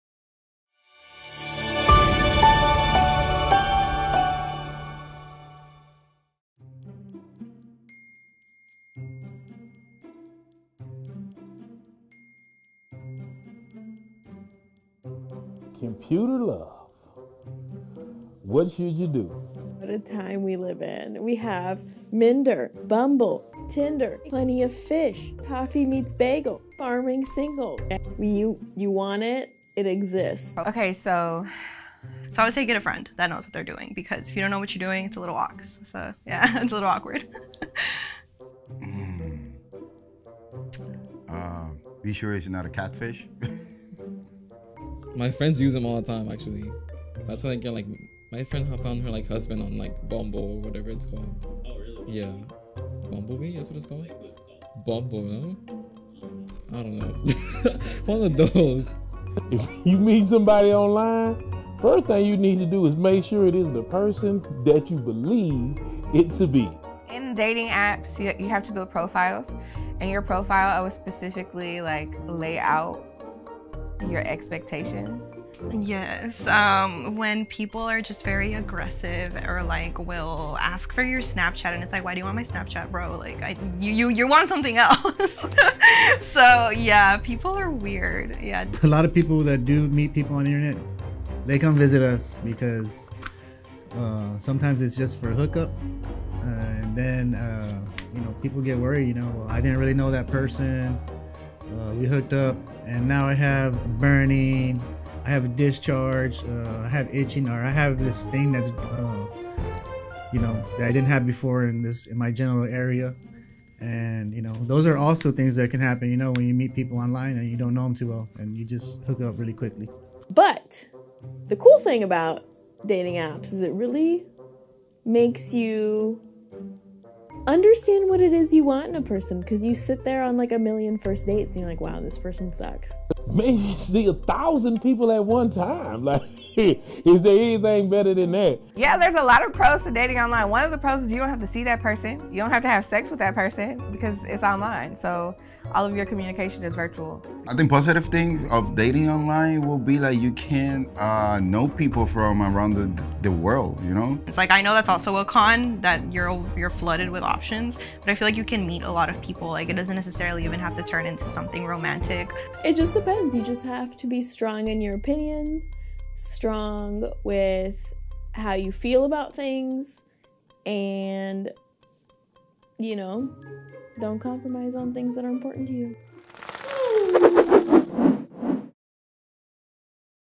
What's the Tea? - EP.2 - Internet Love
There is no judging coming from our Bee Busy Team. In this episode, we discuss our thoughts and suggestions regarding online dating.

Keep following for more. We keep the reactions real and connected!

About What’s the Tea: Simple but sweet stories on real answers and responses to questions that are pretty hard to answer.